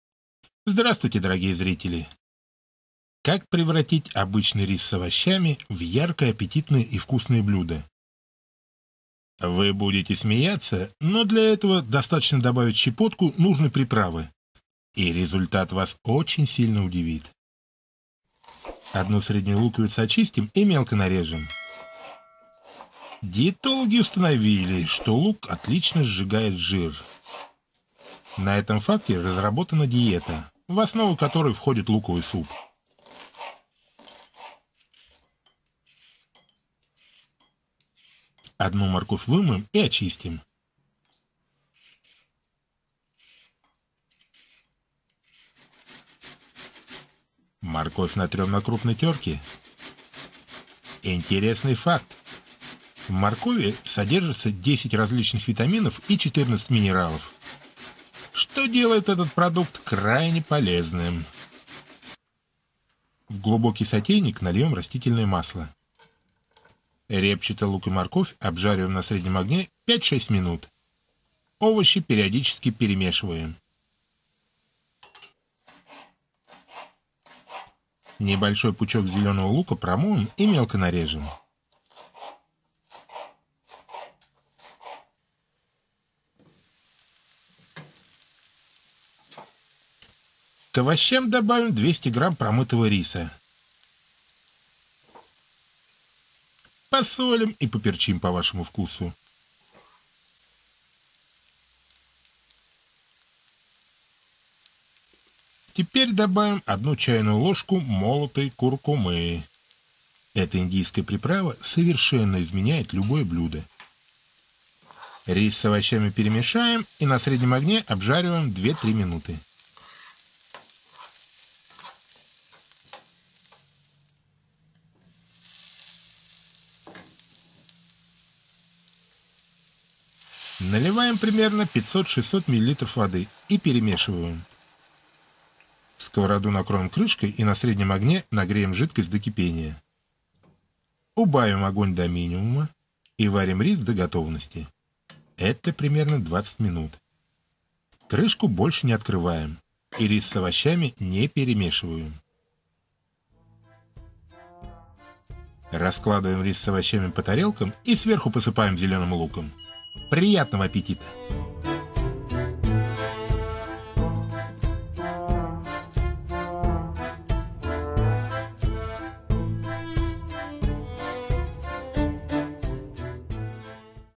Самый вкусный рис с овощами (Субтитры)
Как превратить обычный рис с овощами в яркое, аппетитное и вкусное блюдо? Вы будете смеяться, но для этого достаточно добавить щепотку нужной приправы и результат Вас очень сильно удивит.

Продукты для рецепта «Рис с овощами»:

• 200 грамм риса,
• 1 морковь,
• 1 луковица,
• 1 чайная ложка куркумы,
• соль,
• черный молотый перец,
• растительное масло,
• небольшой пучок зеленого лука.

Как приготовить рис с овощами.

1 среднюю луковицу очистим и мелко нарежем.
1 морковь вымоем и очистим.
Морковь натрем на крупной терке.
В глубокий сотейник нальем растительное масло. Репчатый лук и морковь обжариваем на среднем огне 5-6 минут. Овощи периодически перемешиваем.
Небольшой пучок зеленого лука промоем и мелко нарежем.
К овощам добавим 200 грамм промытого риса, посолим и поперчим по вашему вкусу. Теперь добавим 1 чайную ложку молотой куркумы. Эта индийская приправа совершенно изменяет любое блюдо! Рис с овощами перемешаем и на среднем огне обжариваем 2-3 минуты.
Наливаем примерно 500-600 миллилитров воды и перемешаем, сковороду накроем крышкой и на среднем огне нагреем жидкость до кипения. Убавим огонь до минимума и варим рис до готовности. Это примерно 20 минут. Крышку больше не открываем и рис с овощами не перемешиваем.
Раскладываем рис с овощами по тарелкам и сверху посыпаем зеленым луком.